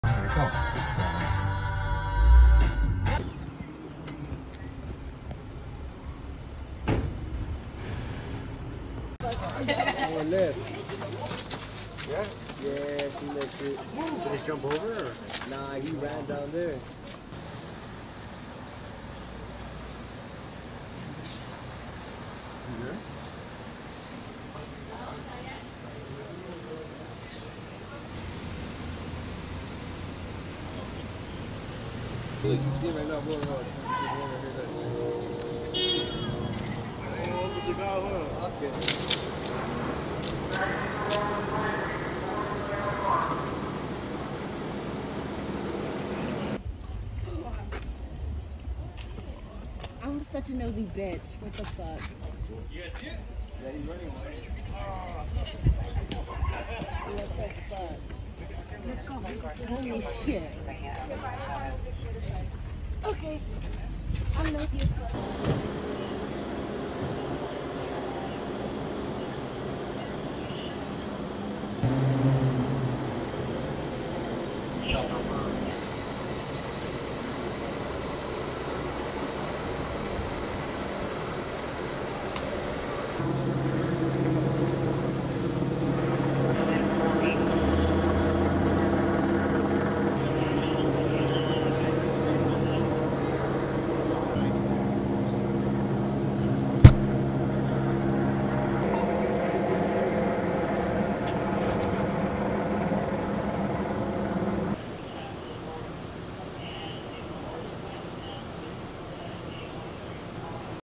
Large Bear In Chatsworth Caught on Camera
LAPD was on scene trying to corner a large bear in Chatsworth. LAPD had the bear in sights with lethal weapons aimed in its direction when they were told by fish and game to clear the area and leave it alone. The bear is still currently in the back property of a business. The incident took place at Superior and DeSoto in chatsworth at 11:20pm